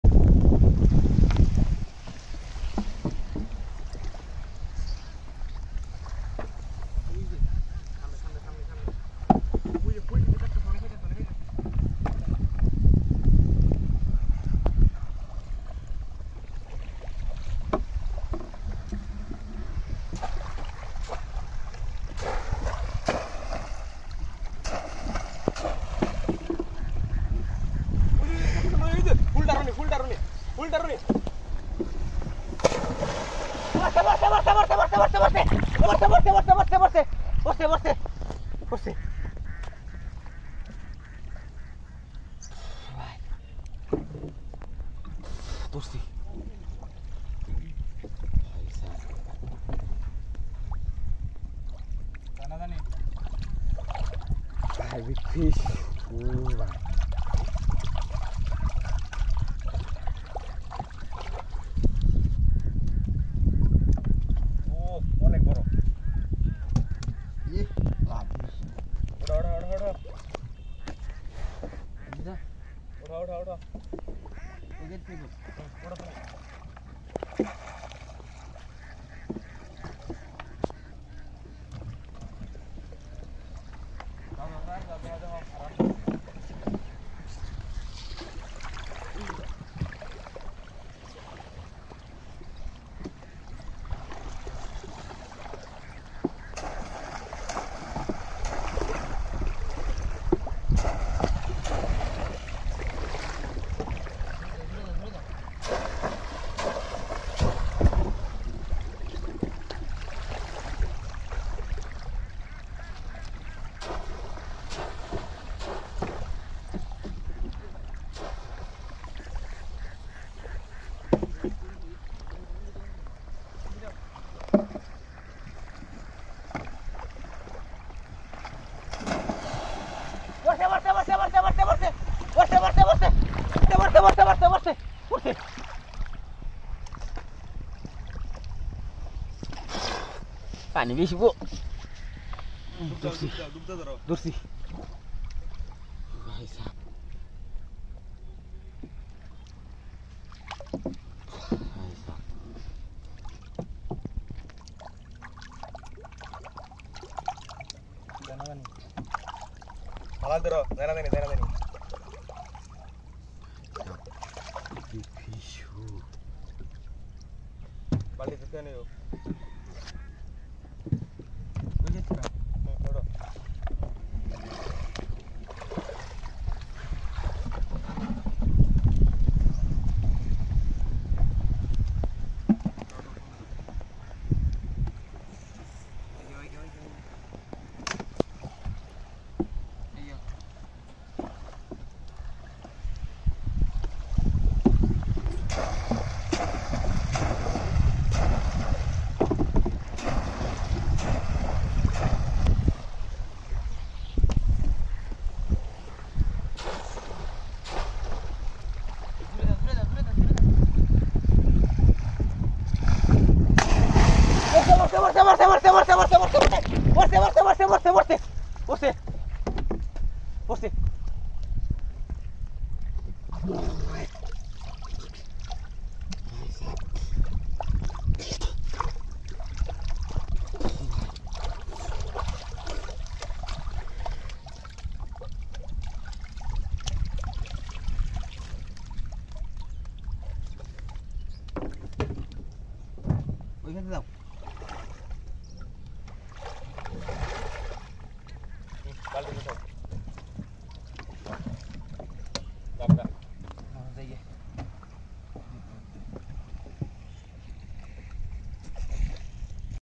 Daily Amazing Polo Fishing With Beautiful Village Boat Flood Water ] Boy Catch Fish By Polo In Bill
enjoy of the fishing video. 🤟🤟🤟🥰🥰

1. @macarem125
2. @kingfishlover1695
3. @amazingfishingpossible6455

thanks my All viewrs
thanks FOR wishing